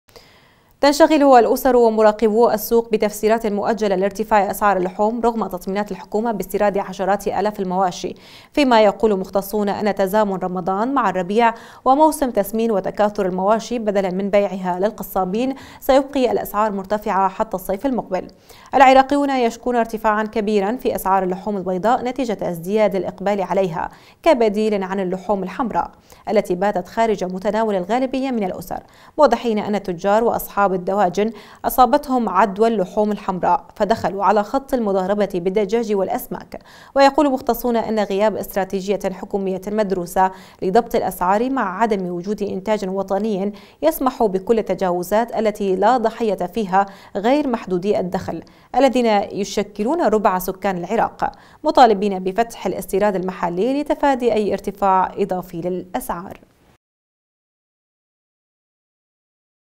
مواطنون يشكون أسعار اللحوم للحكومة
تنشغل الأسر ومراقبو السوق بتفسيرات مؤجلة لارتفاع أسعار اللحوم رغم تطمينات الحكومة باستيراد عشرات الاف المواشي، فيما يقول مختصون ان تزامن رمضان مع الربيع وموسم تسمين وتكاثر المواشي بدلا من بيعها للقصابين سيُبقي الأسعار مرتفعة حتى الصيف المقبل، العراقيون يشكون ارتفاعا كبيرا في أسعار اللحوم البيضاء نتيجة ازدياد الإقبال عليها كبديل عن اللحوم الحمراء، التي باتت خارج متناول الغالبية من العأسر، موضحين أن التجار وأصحاب الدواجن أصابتهم عدوى اللحوم الحمراء فدخلوا على خط المضاربة بالدجاج والأسماك، ويقول مختصون ان غياب استراتيجية حكومية مدروسة لضبط الأسعار مع عدم وجود انتاج وطني يسمح بكل التجاوزات التي لا ضحية فيها غير محدودي الدخل الذين يشكلون ربع سكان العراق؛ مطالبين بفتح الاستيراد المحلي لتفادي أي ارتفاع اضافي بالأسعار.